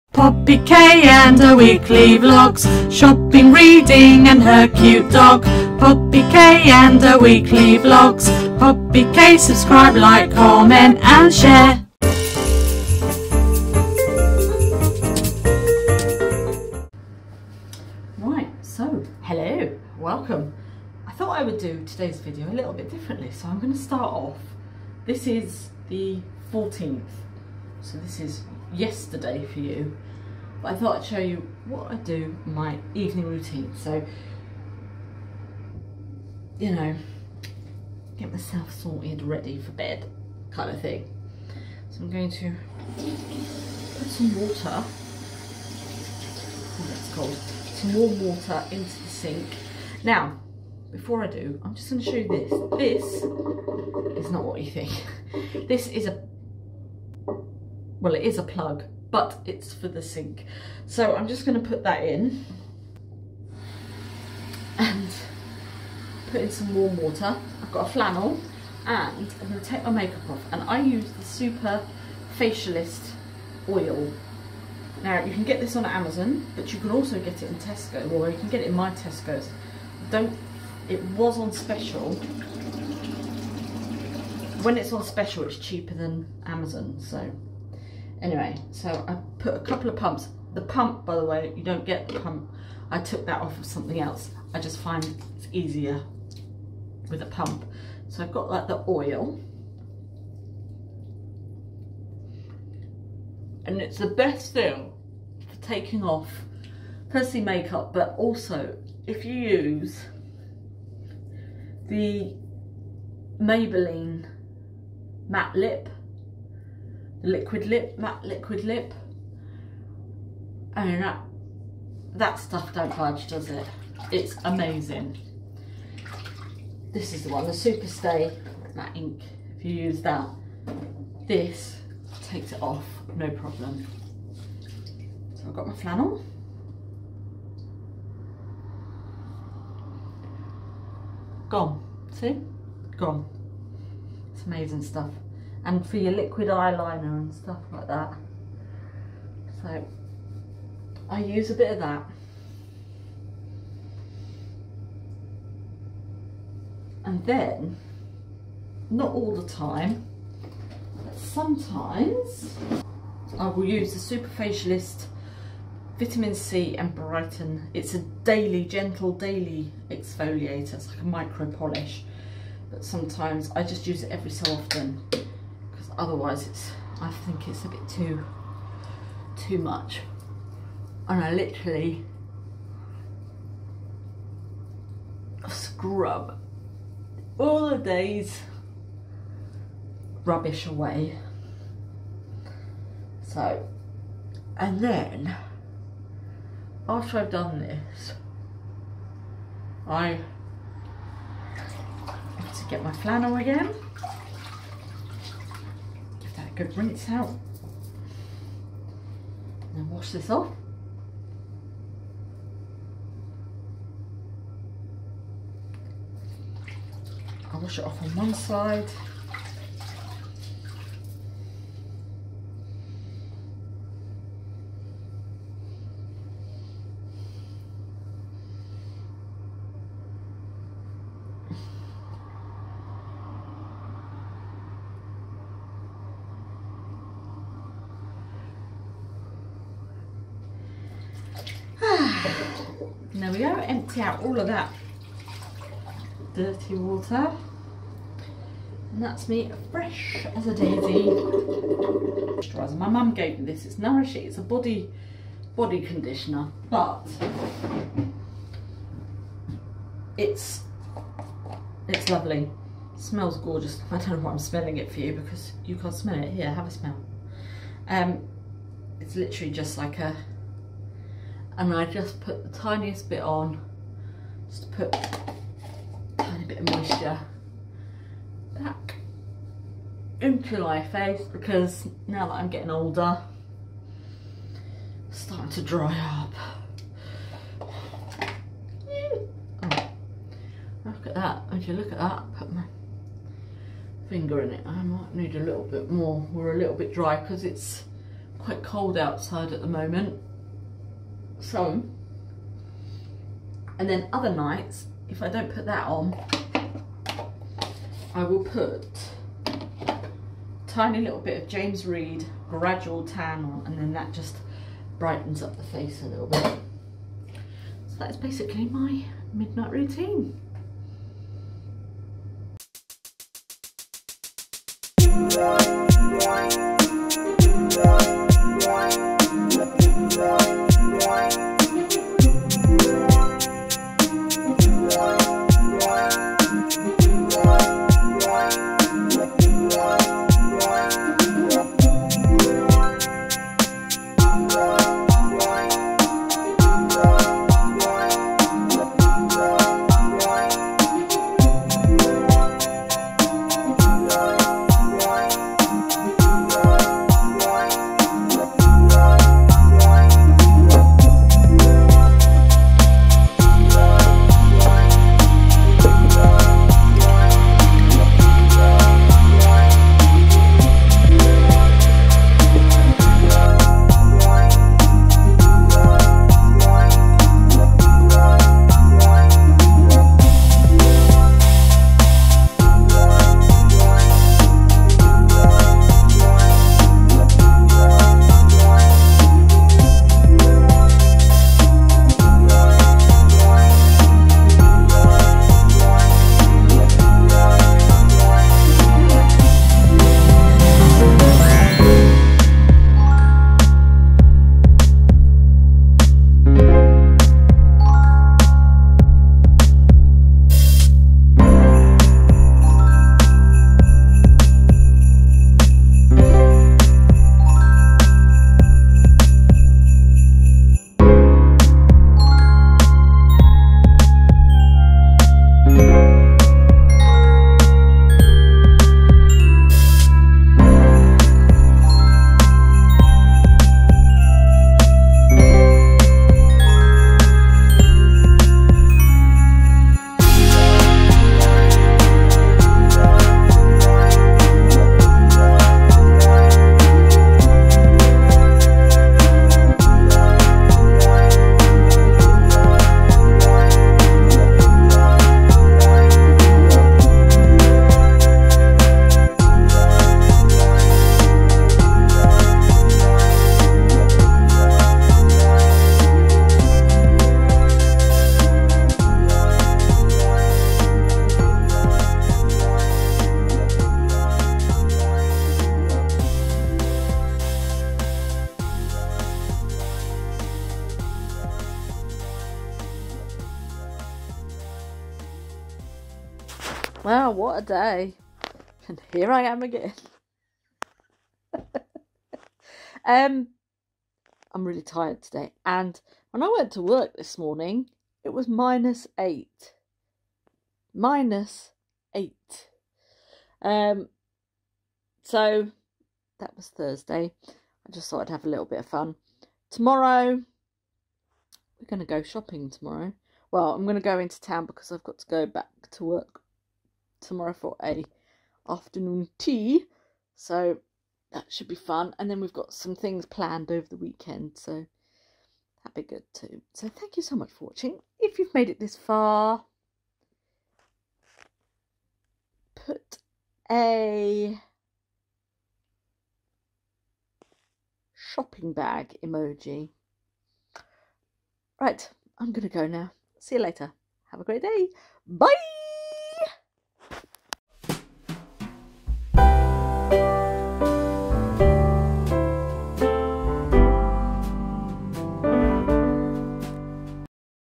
Vlogmas 15 - Come to Work With Me
Welcome to Vlogmas, Spend the day with me and see what I get up to.  Come to work with me!!